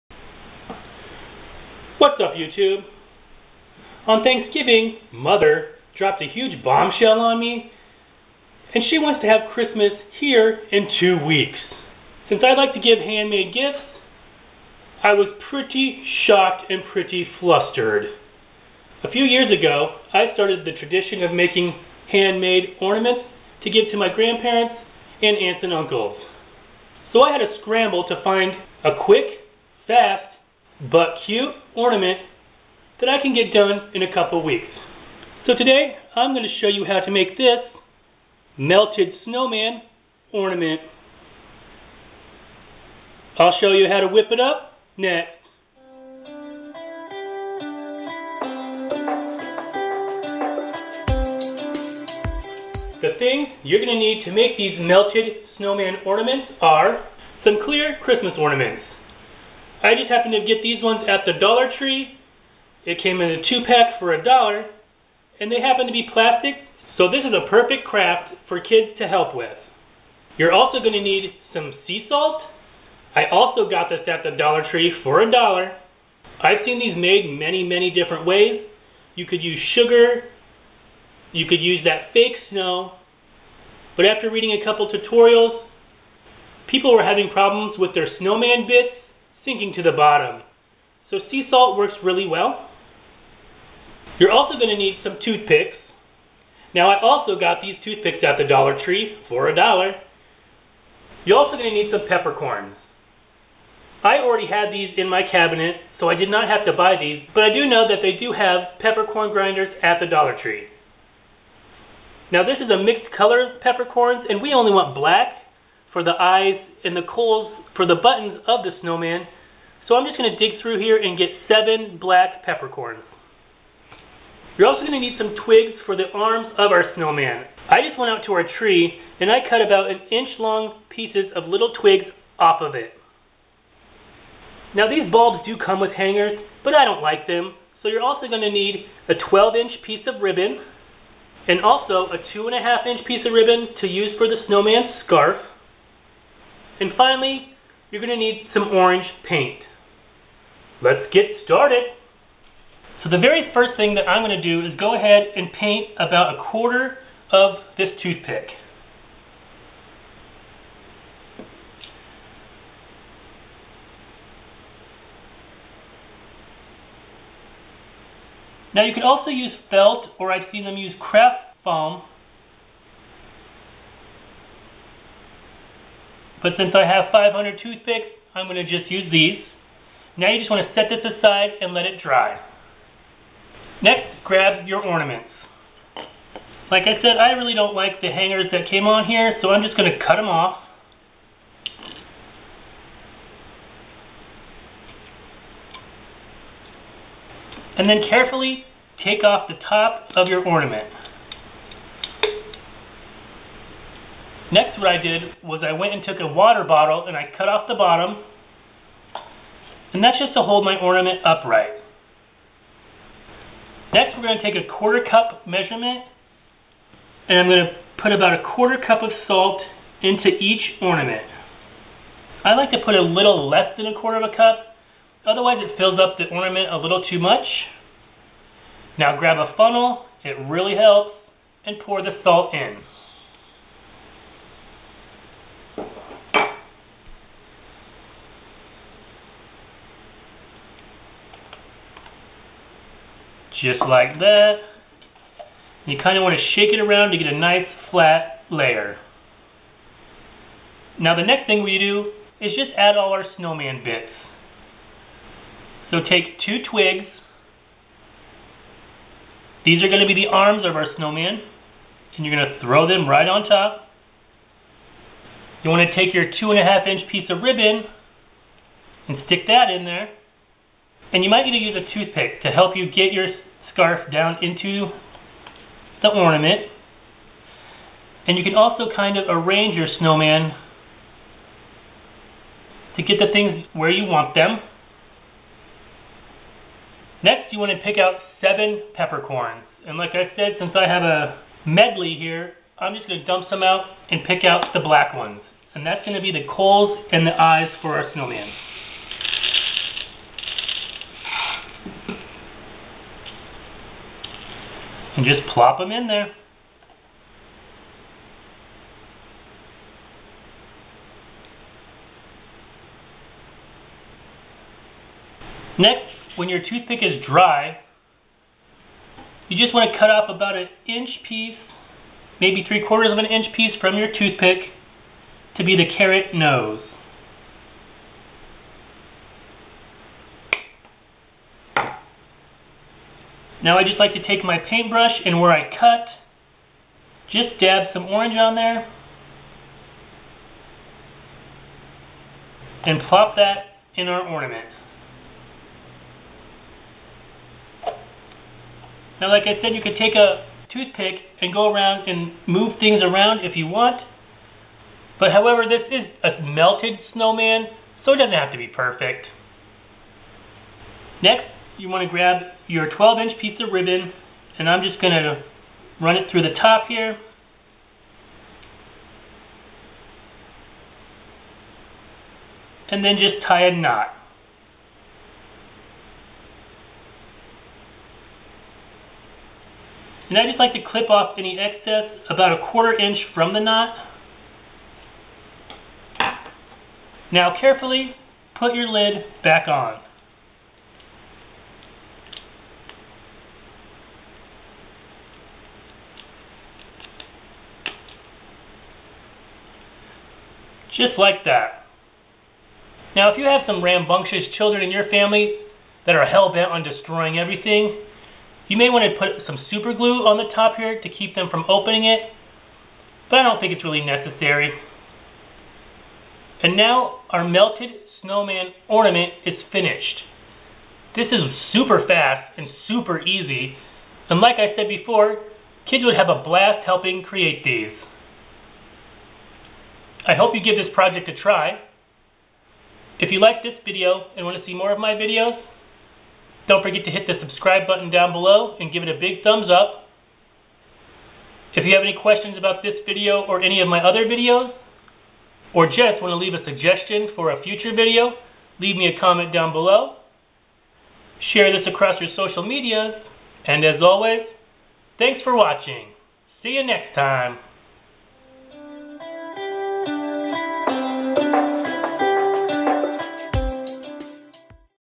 Melted Snowman Ornaments
To I will show you how to whip up these fast and easy melted snowman ornaments for the upcoming holidays!  All the things you need to make these can be found at your local Dollar Tree.  Since I used clear plastic ornaments, these are a perfect project for the kids!